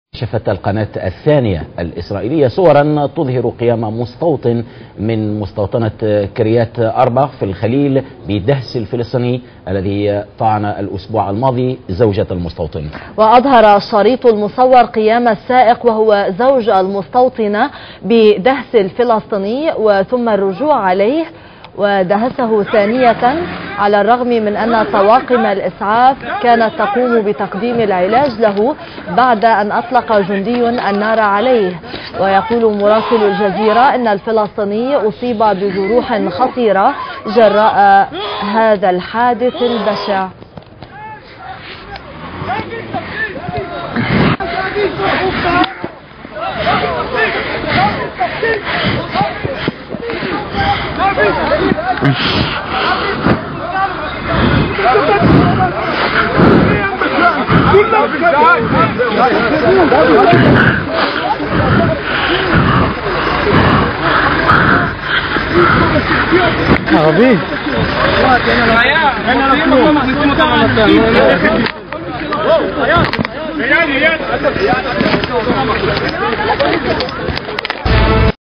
(1) مستوطن صهيونى يدهس فلسطينى بسيارته ٢ ديسمبر ٢٠٠٩
(EN) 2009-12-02 A zionist settler run over a Palestinian (Wassim Ussama Souda) under the eyes of the soldier,
The Palestinian has been shot by a soldier, after which a car, apparently driven by a settler, ran over the wounded Palestinian, twice, with Israeli soldiers all around.

(FR) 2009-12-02 Un colon sioniste ecrase un Palestinien (Wassim Ussama Souda) sous les yeux des soldats,
Le Palestinien a été blessé par un soldat, après quoi une voiture, apparemment conduite par un colon, s'est déroulée sur le Palestinien blessé, deux fois, avec des soldats israéliens tout autour.

(AR) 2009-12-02 .مستوطن يدهس فلسطينيا بحضرة جنود